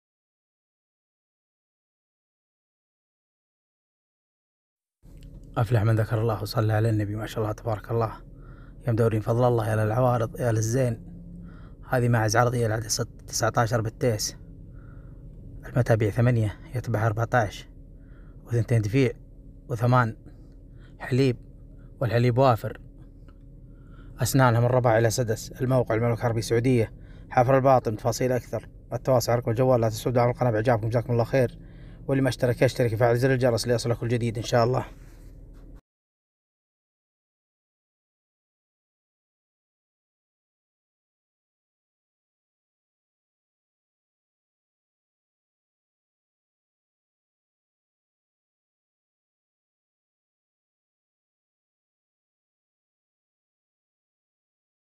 ماعز عوارض فاخره عدد ١٩ بالتيس يتبعها ١٤ للبيع رقم واتساب 0555865846
حراج على ماعز عوارض فاخره جروم وديود خصم
العدد ١٩ بالتيس
المتابيع ٨ معها ١٤ عيالها منادي
٢ دفيع. و ٨ حليب. والحليب وافر  مركنه خصصم
الأسنان رباعيه وسدس شبب شرط.
الموقع /   حفرالباطن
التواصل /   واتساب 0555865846